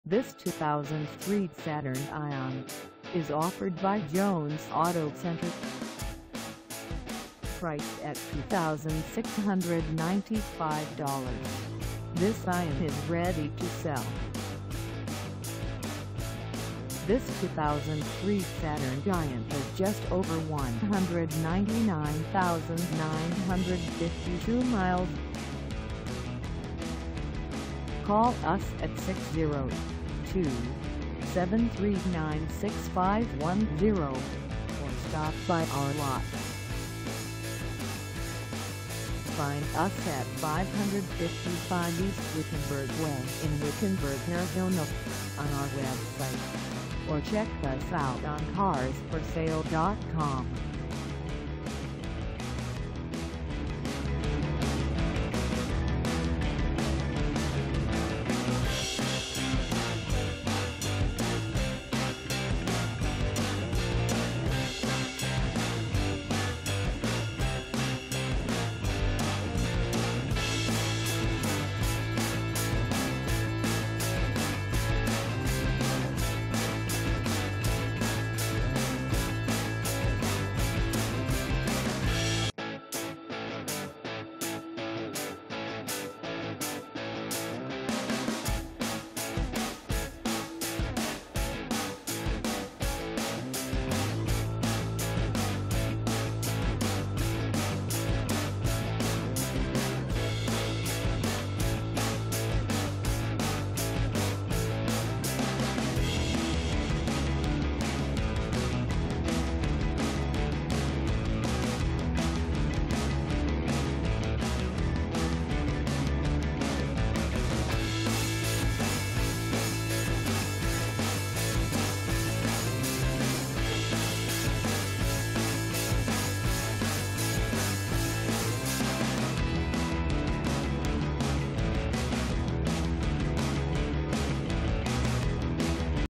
2003 Saturn Ion 2 4dr Sedan for sale in Wickenburg, AZ 85390
This 2003 Saturn Ion 2 4dr Sedan is for sale in Wickenburg, AZ 85390 at Jones Auto Center.

Engine: 2.2L I4
Transmission: Automatic 5-Speed
Color: Bright Blue
Mileage: 199952
Contact Jones Auto Center 555 E Wickenburg Way Wickenburg, AZ 85390 to test drive this 2003 Saturn Ion 2 4dr Sedan today.